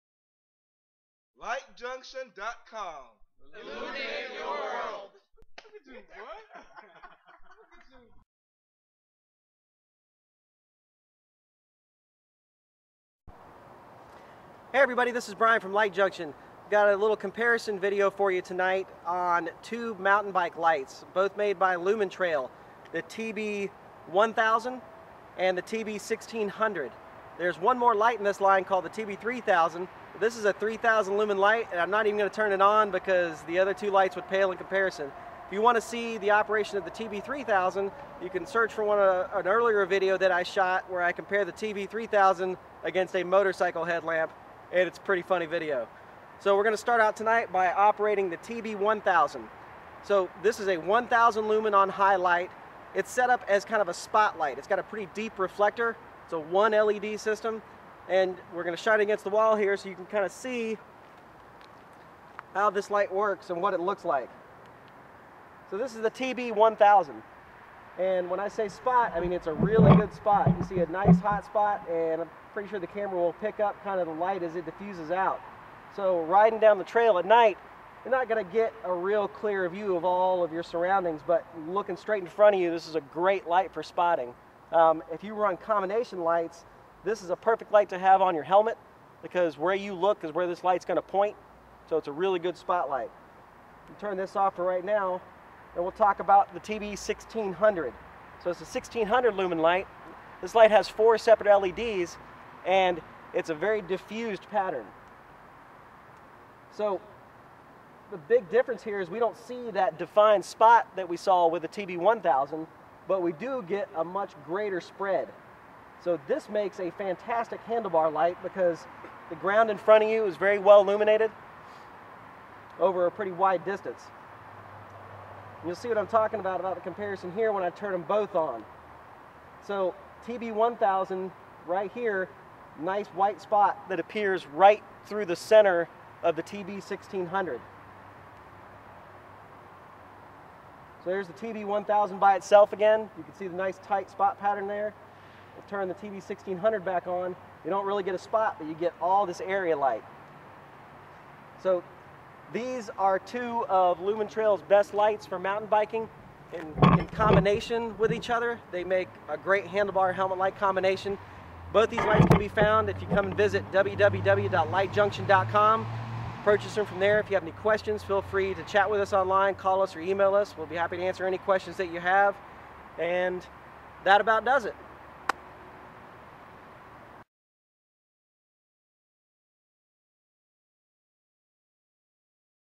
Lumintrail TB1000 & TB1600 LED bike light comparison review
This is a review comparison of the Lumintrail TB-1000 and TB-1600 LED bicycle lights.  Designed with trail riding at night in mind, both of these lights have characteristics to make your night ride more exciting.  Visit LightJunction.com to check out these and all the other exciting bike lights.